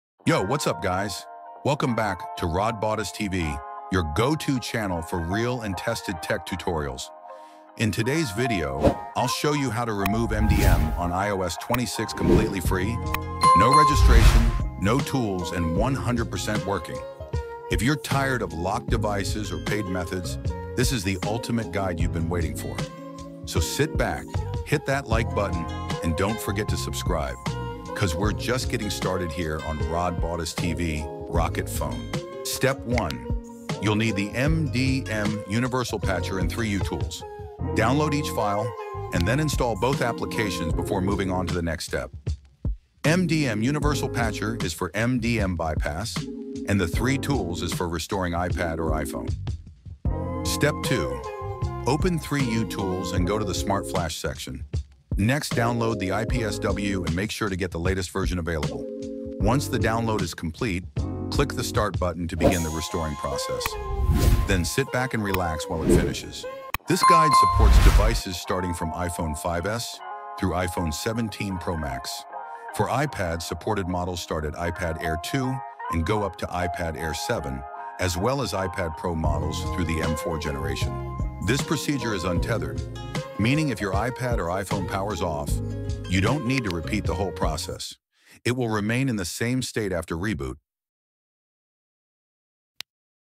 MDM Bypass ios 26
In this video, I’ll show you the complete list of supported Apple devices for the latest iOS 26 and macOS setup process.
This includes iPhone models from iPhone 5s all the way to iPhone 17 Pro Max, and iPads from iPad Air 2 up to iPad Air 7 and iPad Pro M4.

If you’re planning to update, restore, or set up your iPhone or iPad — make sure your device is on the list!

3utools is here
3u.com